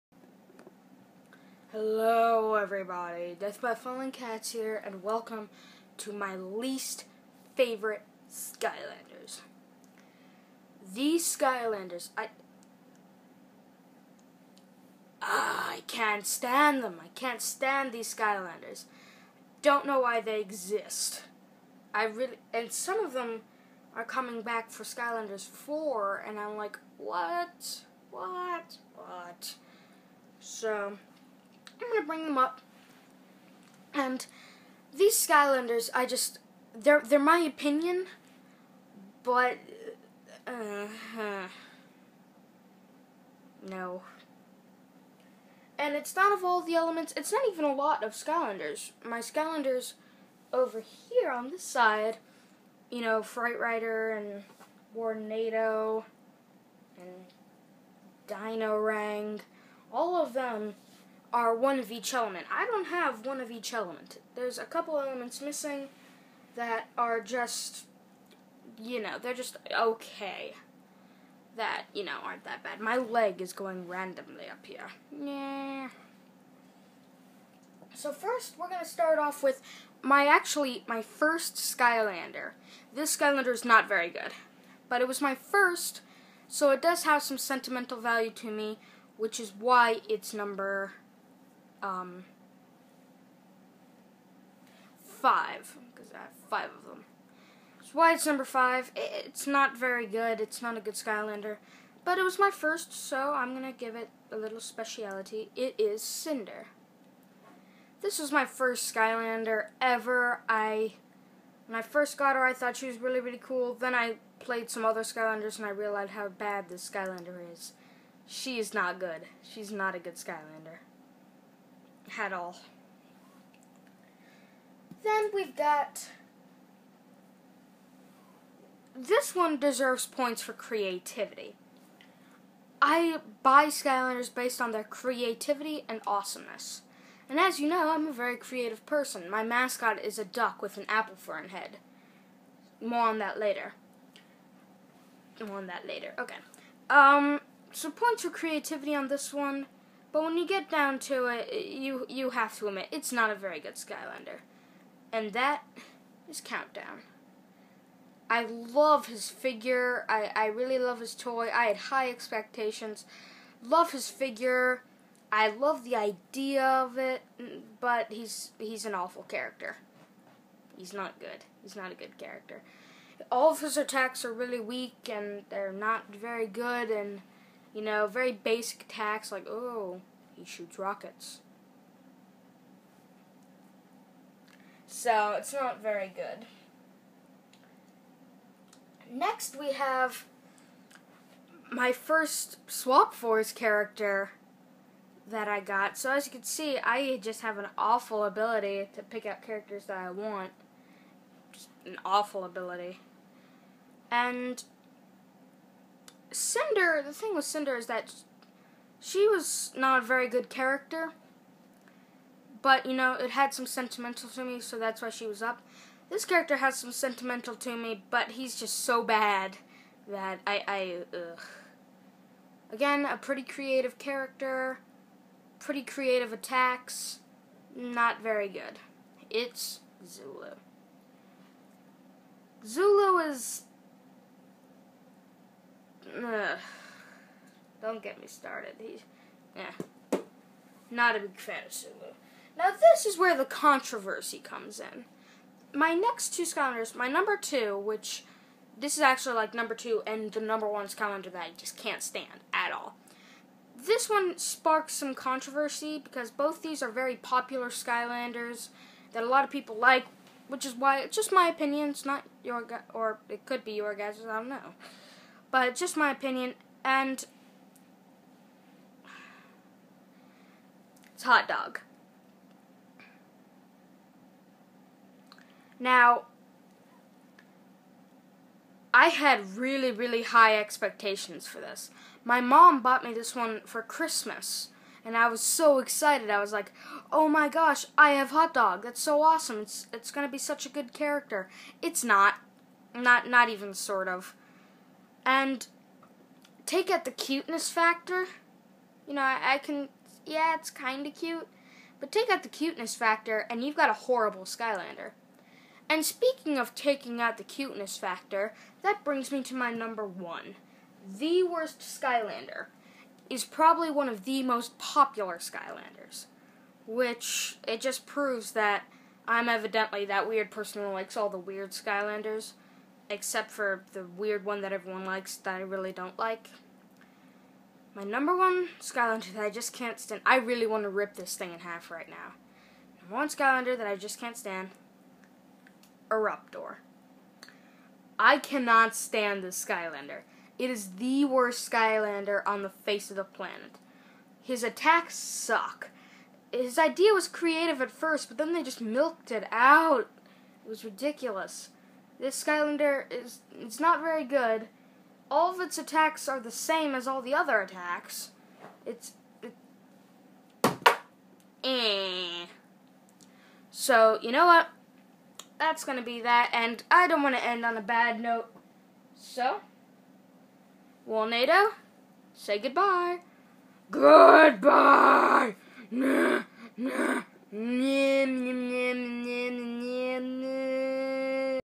My top five Skylanders ever
I say they suck but I really mean they are the best. I collect skylanders for a living, not creepy, right?